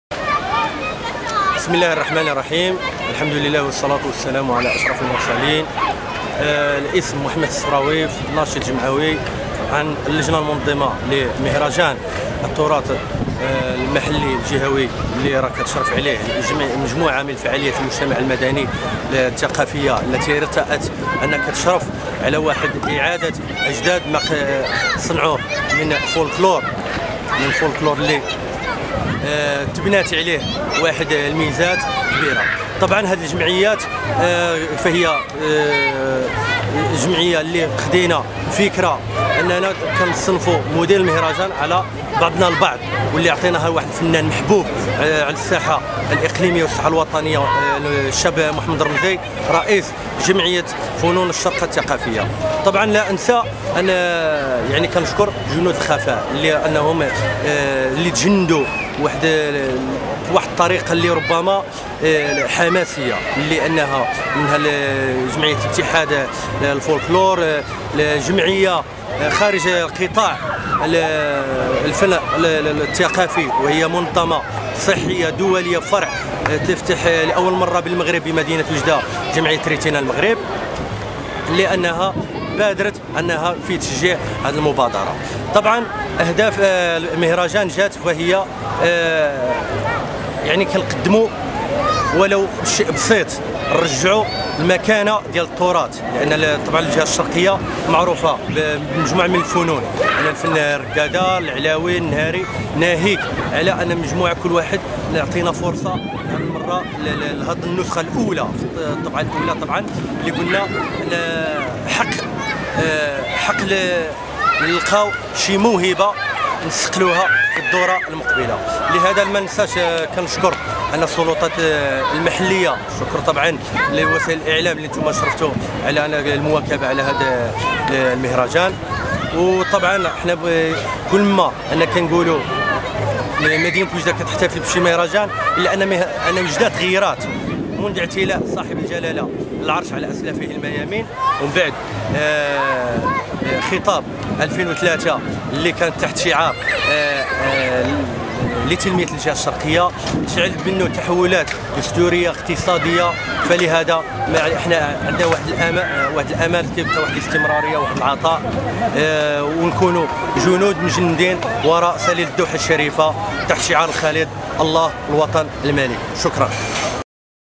تصريح محمد الصفراوي حول المهرجان الثقافي للتراث المحلي بوجدة لوجدة الوطنية
تصريح محمد الصفراوي عن اللجنة المنظمة للمهرجان الثقافي الجهوي للتراث المحلي في دورته الأولى بوجدة والذي نظمته جمعية فنون الشرق للثقافة بالشراكة مع: جمعية المواهب، جمعية الفن الفلكلوري، اتحاد جمعيات الفلكلور، جمعية فن أحيدوس، جمعية لعوافي، جمعية ريتينا المغرب بساحة زيري بن عطية أيام 11-12-13 يونيو بوجدة